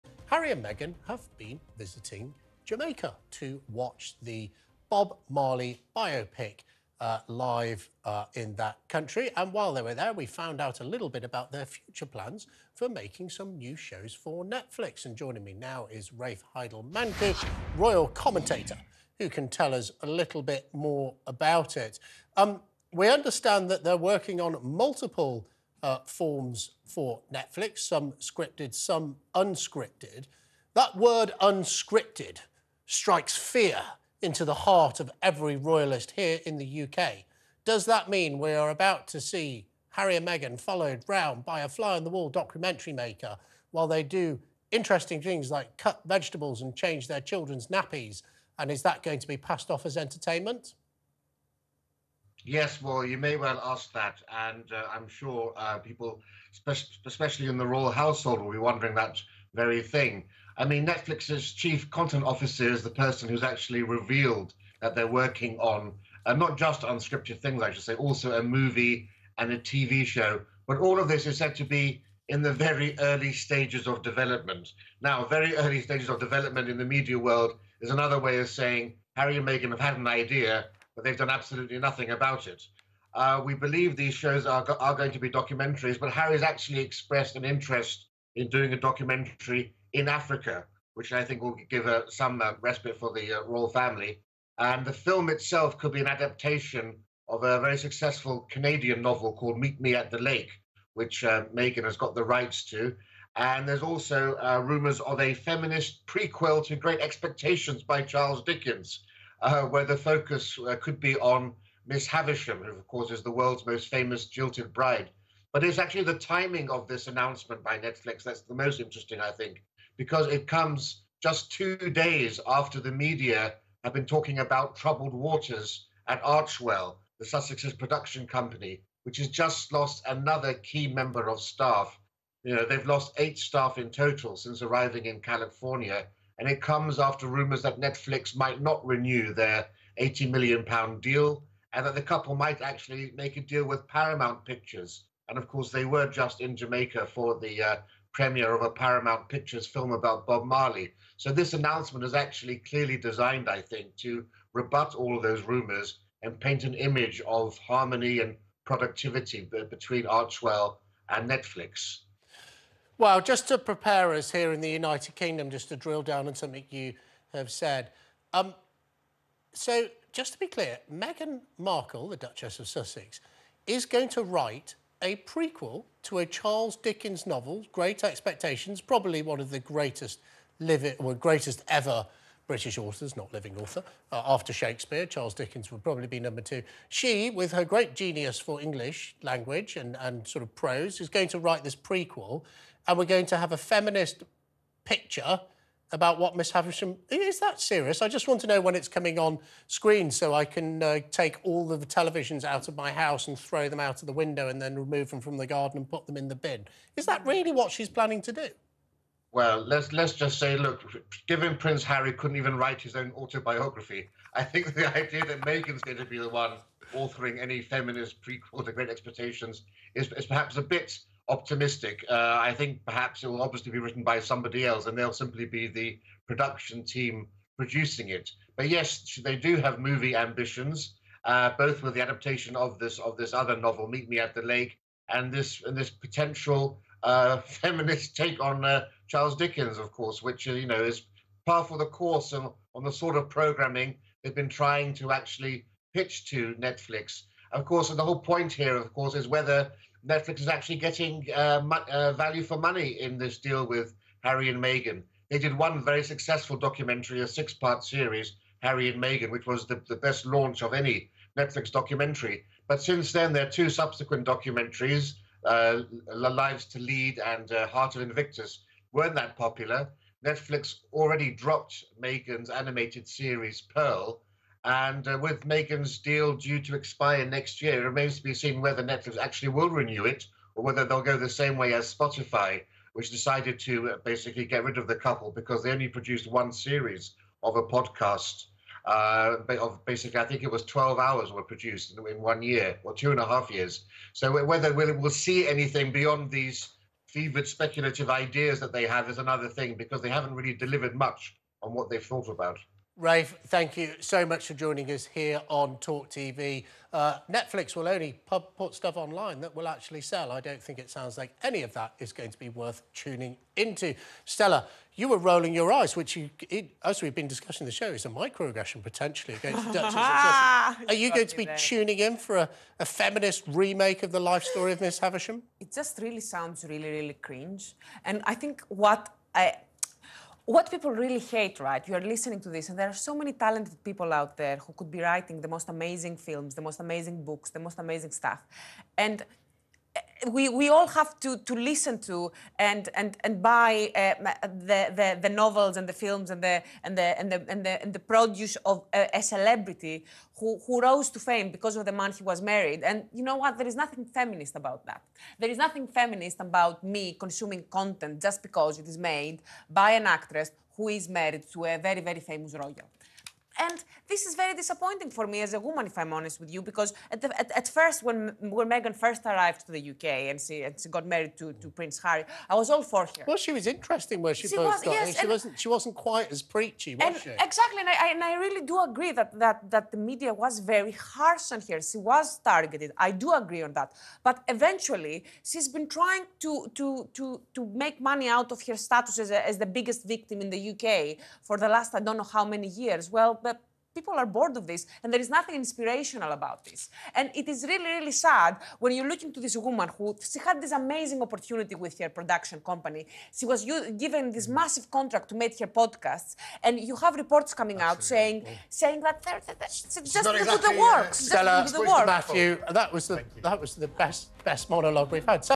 Royal Commentator SLAMS Meghan Markle Over Feminist Prequel Of Great Expectations
Royal commentator Rafe Heydel-Mankoo slams Meghan Markle for her latest Netlfix quest to author a feminist prequel of Charles Dickens' ‘Great Expectations.’

This comes after Prince Harry and Meghan have been hit with a fresh blow after their Archewell Productions manager walked out.

The company, which boasts a collaboration with Netflix, was launched in 2020 by the Sussexes in a bid to create "programming that informs, elevates, and inspires".

This month, Archewell manager Bennett Levine - who worked on their Netflix docuseries Harry and Meghan - confirmed he was quitting, after less than two years in the role.

It follows a string of staff leaving the company, amid rumours circulating the Duke and Duchess are about to have their Netflix deal scrapped.

Rafe says: “I think it will obviously be written by somebody else, and they will simply be the production team.”